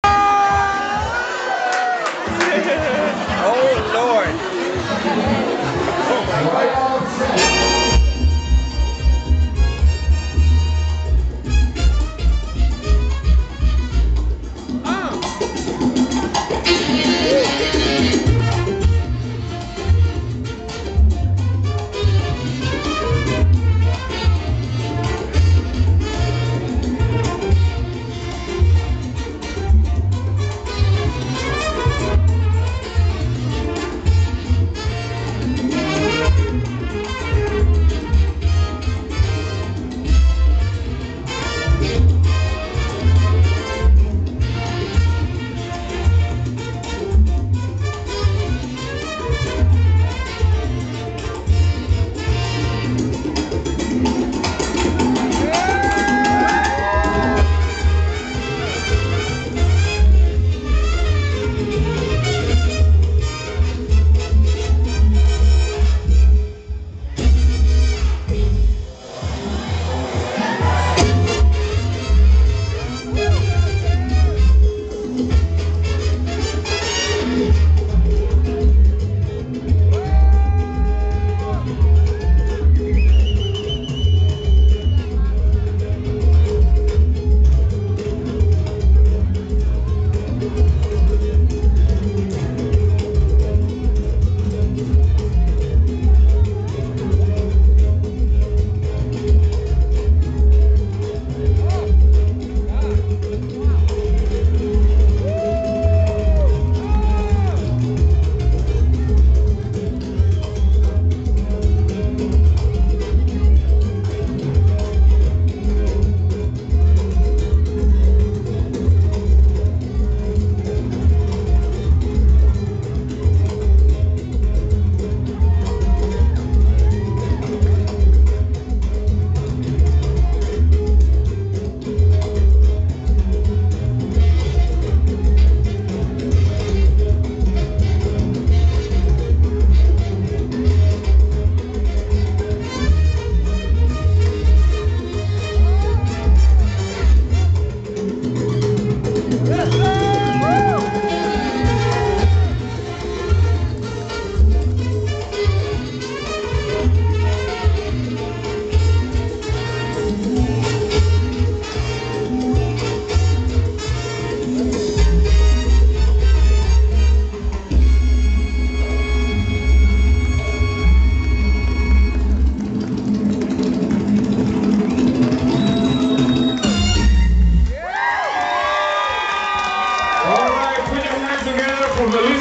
Melissa Rosado performing at the NJ Congress Pre-Party @ Perle, April 2011
Crossover shoes by: Burju (Very cute!)
PLEASE DO NOT USE PROFANITY WHEN COMMENTING, THANK YOU.